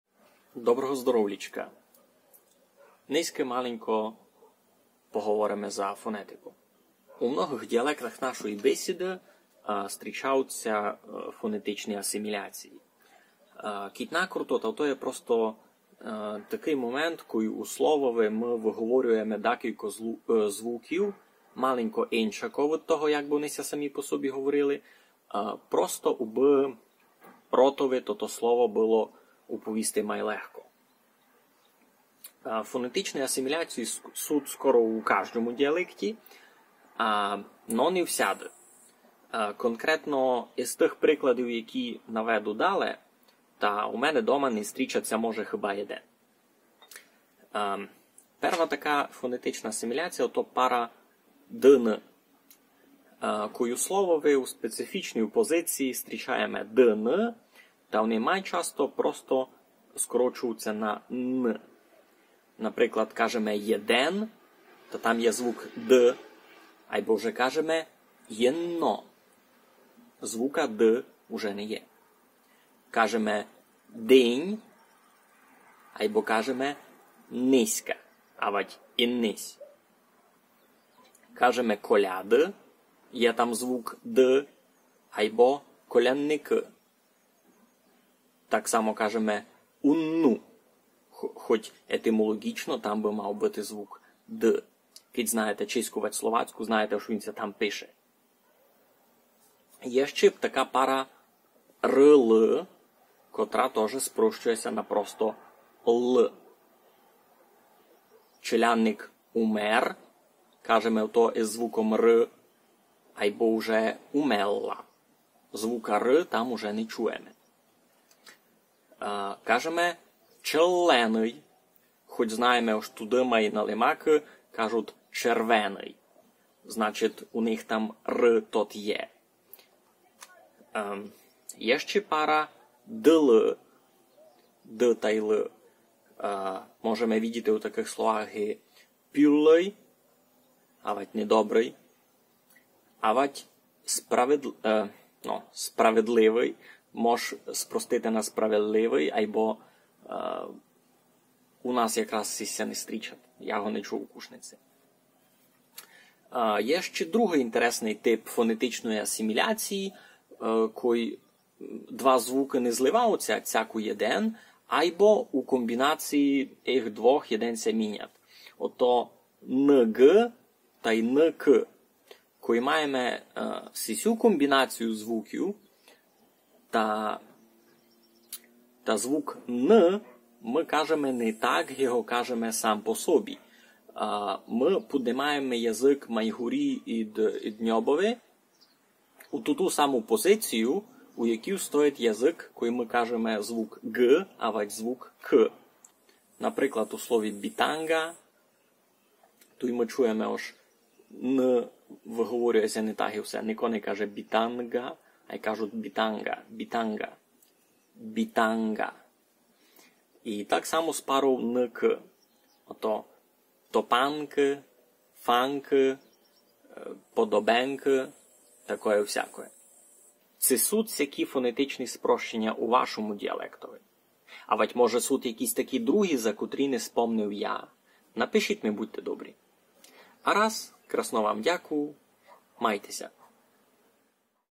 Умелла ци умерла? Як тото писати? | Карпаторусинська фонетика
📚 У сюм відео розказуву за процесы фонетичного спрощіня (асимілації) помежи дакотрыми парами приголосных звукӯв: ДН, ДЛ, РЛ, НҐ, НК

Ба ци люблят ся вам сисї відеа? Кидь айно, можете ня пуддержати на патреонови, авадь шмарити исе відео цімборам! Дякуву!